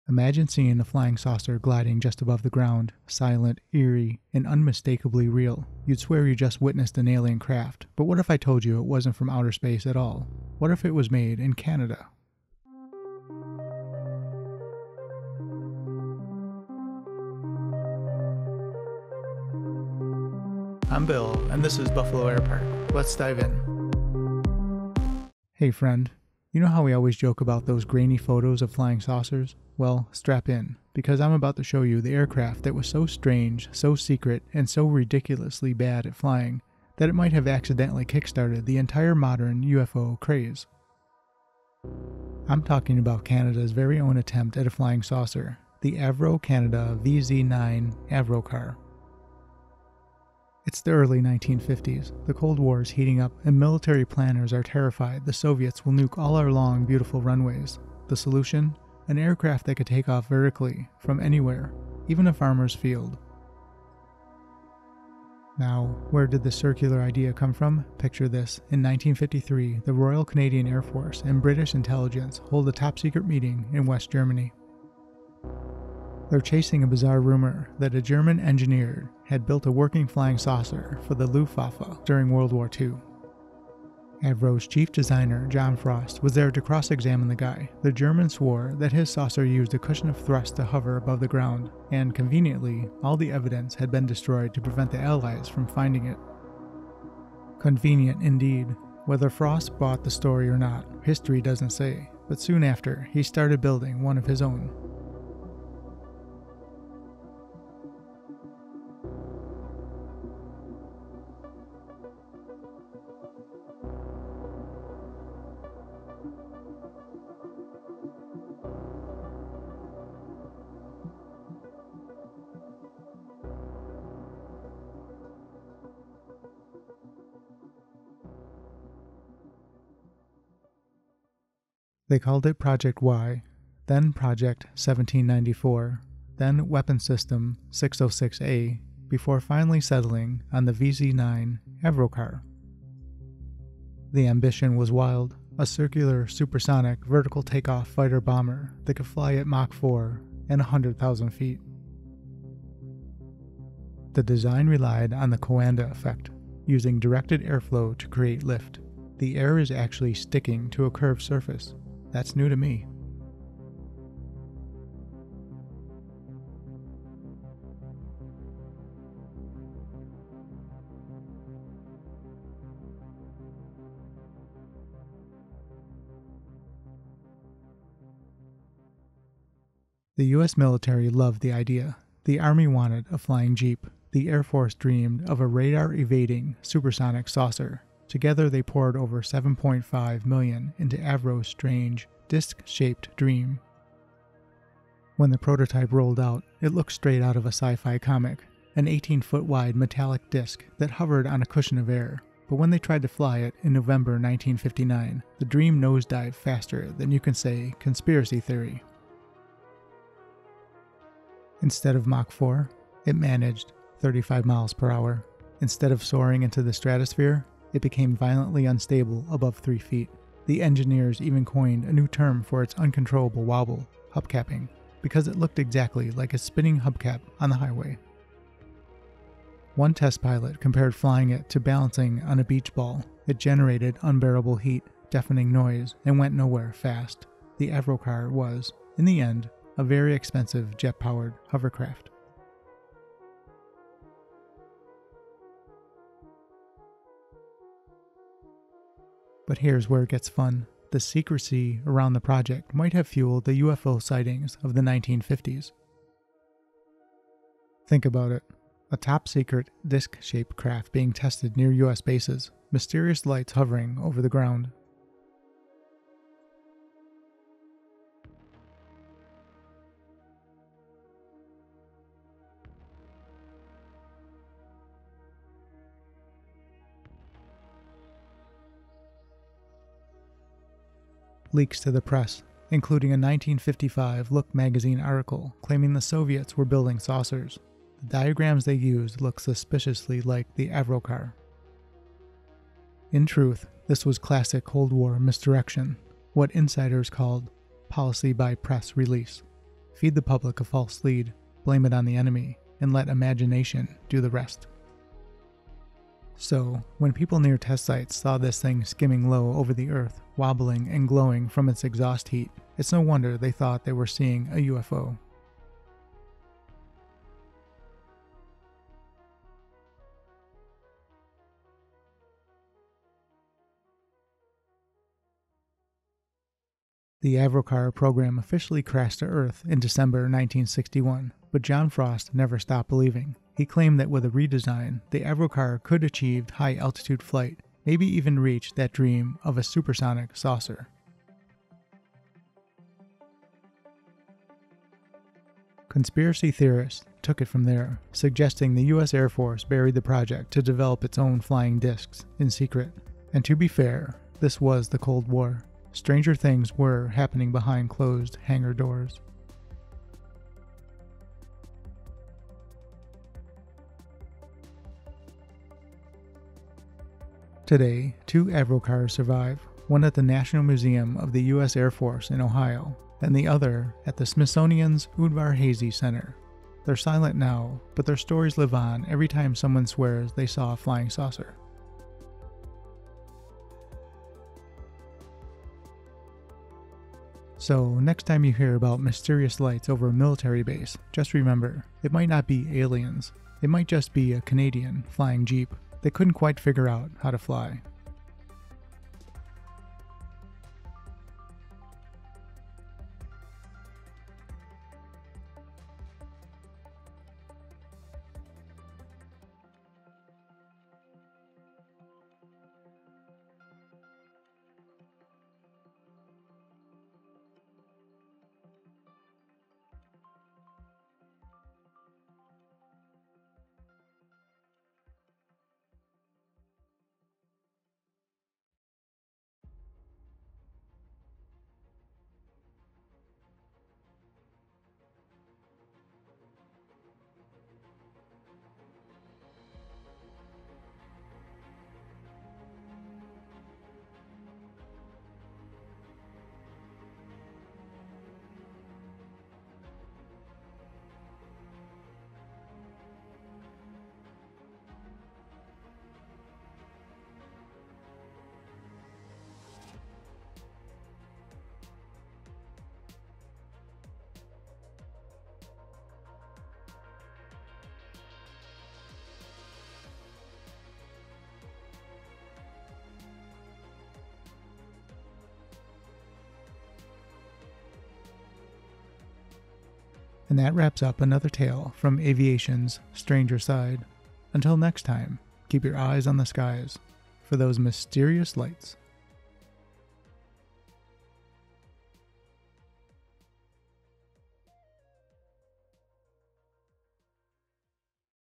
Avrocar: Canada's Flying Saucer That Couldn't Fly Straight
They called it Canada’s answer to the flying saucer — a top-secret Cold War project so bizarre it might’ve fueled the entire UFO craze. In the 1950s, engineers at Avro Canada tried to build a disc-shaped jet that could take off vertically, hover, and fly at supersonic speeds. What they ended up with was an unstable, overheated, spinning nightmare known as the Avrocar.

Join me as we uncover the story of the flying saucer that couldn’t fly straight — a Cold War experiment that blended ambition, secrecy, and science fiction in equal parts.